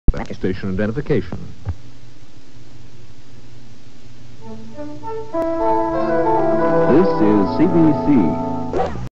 CBC Gem Station Identification from 1983
Pulled from Betamax tape of January 23rd 1983 broadcast of CKPG (CBC)